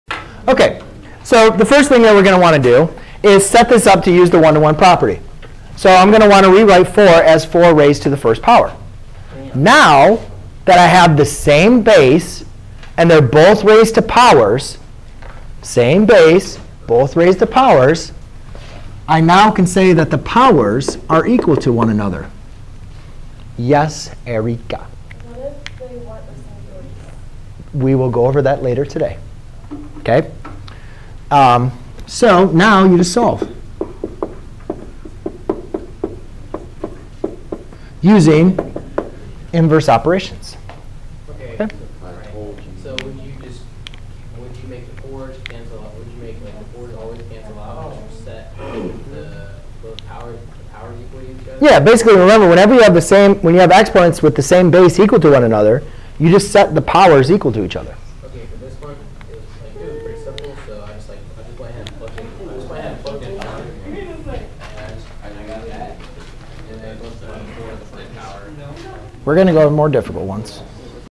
Using the one to one property to solve a basic exponential equation
👉 Learn how to solve exponential equations with the same base. An exponential equation is an equation in which a variable occurs as an exponent. To solve an exponential equation, we make the base of both sides of the equation to be equal so that we can then equate the exponents. If the base of both sides of the equation is equal, we then equate their respective exponents and then solve them.